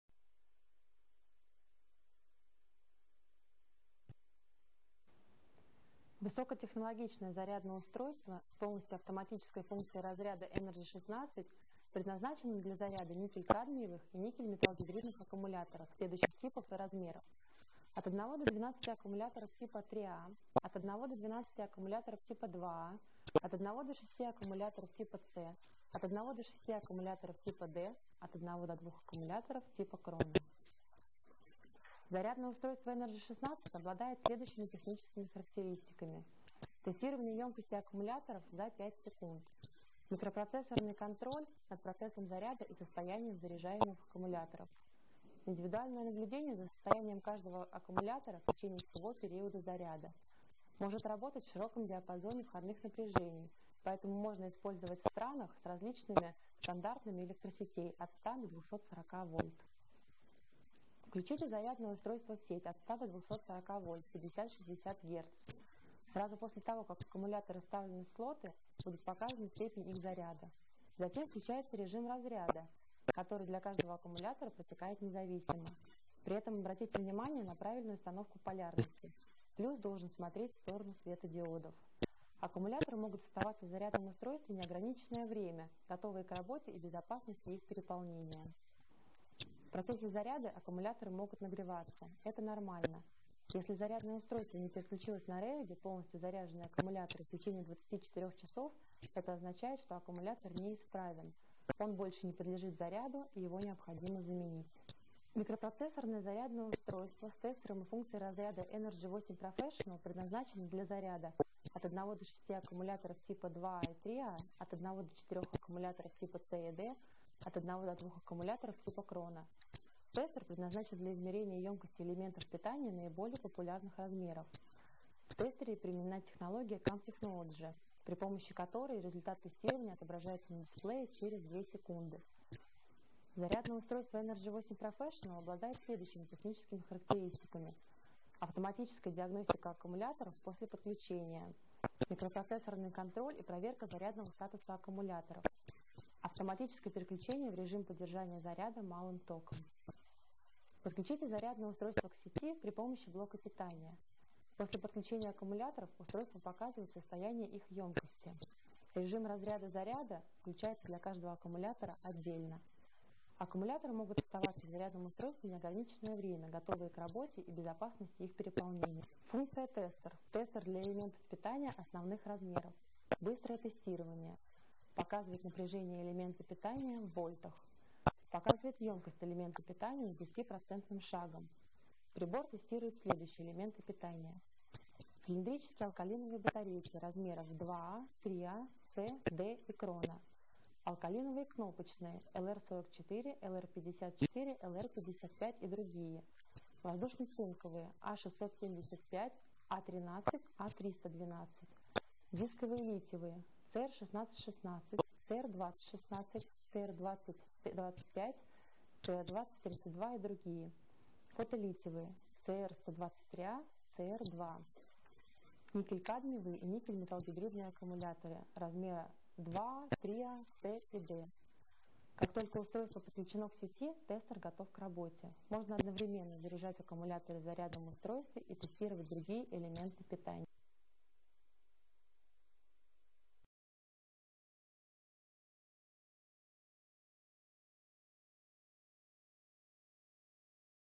ANSMANN ENERGY battery charges
High-tech charger with a fully automatic function of the discharge is designed to recharge nickel-cadmium (NiCd) and nickel-metalgidridnyh (NiMH) batteries of all sizes. During operation for more than 10 years of failures was not. The scheme does not have any regulatory elements, charging current is set automatically. Charger capable of recharging one battery and a battery of several batteries. This charging current varies slightly.